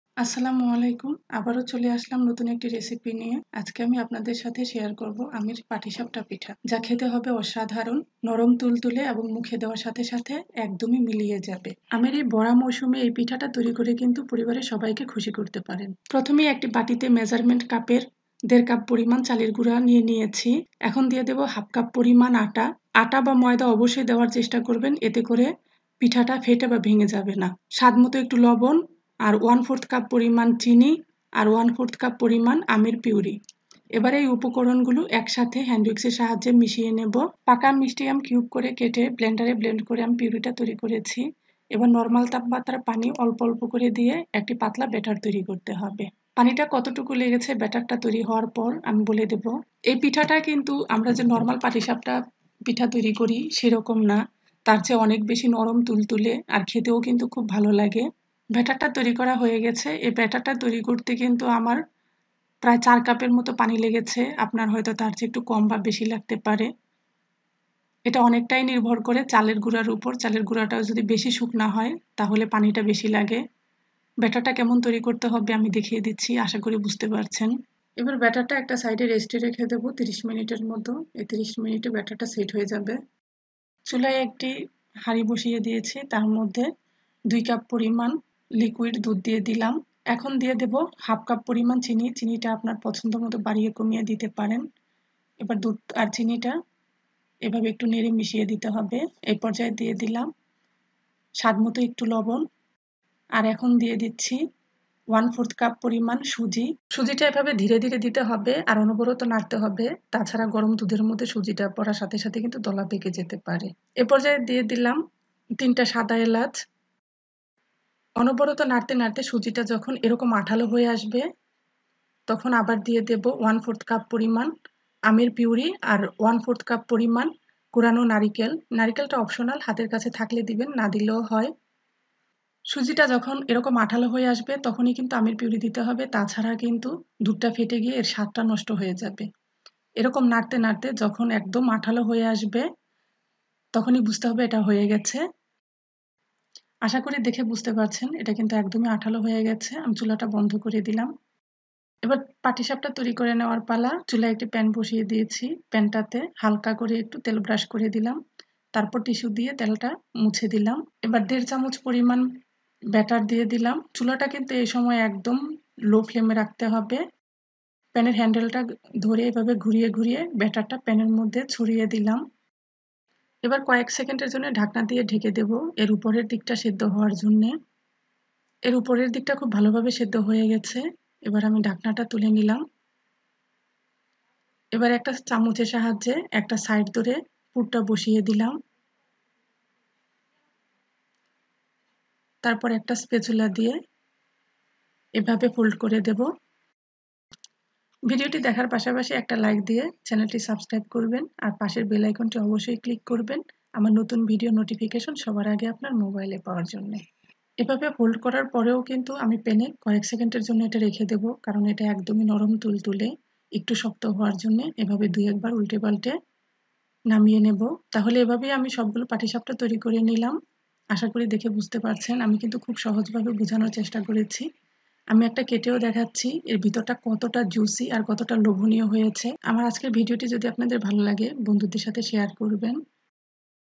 ভীষন মজাদার আমের পাটিসাপটা পিঠা । Amer Patisapta ( টিপস সহ ) | Paka amer Patisapta | Pitha Recipe
আমের  পাটিসাপটা একধরনের পিঠা যা বাংলাদেশ এবং ভারতের পশ্চিমবঙ্গ রাজ্যে জনপ্রিয়। এটি ময়দা, চালের গুড়ো, চিনি, দুধ, ক্ষীর,পাকা আম, নারকেল ইত্যাদি দিয়ে তৈরী করা হয়।পাটিসাপটার পুর হিসেবে নারকেল এবং সুজি ও পাকা আম  - দুইই ব্যবহার করা হয় ।